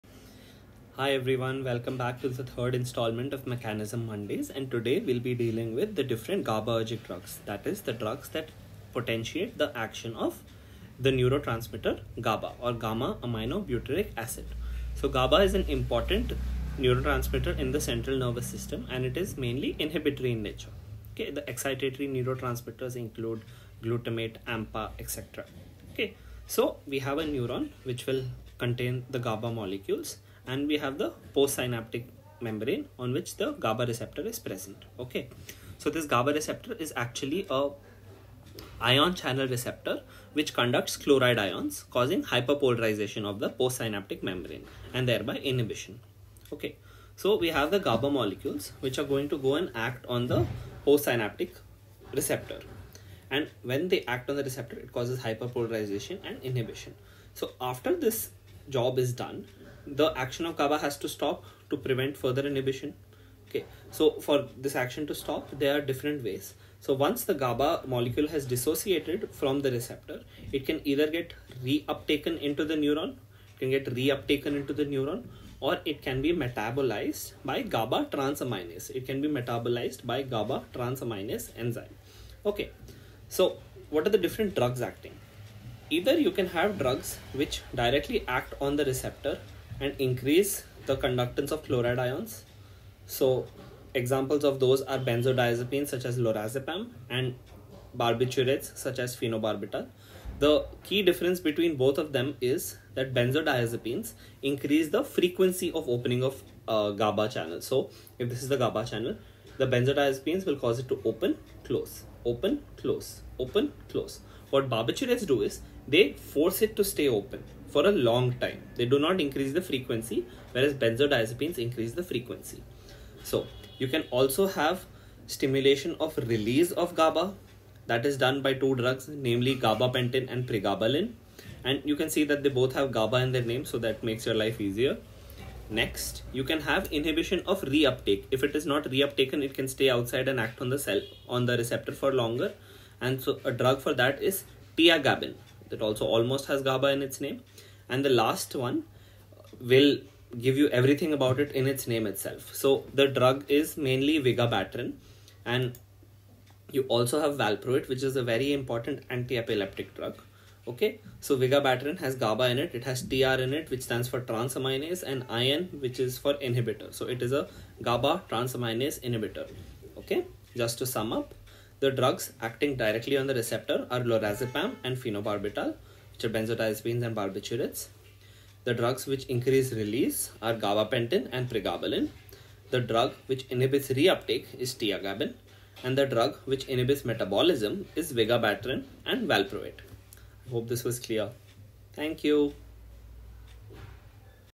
Mechanism Mondays: GABAergic drugs
Today we deal with the various GABAergic drugs and how they act.

Can you name newer GABAergic drugs in the comments?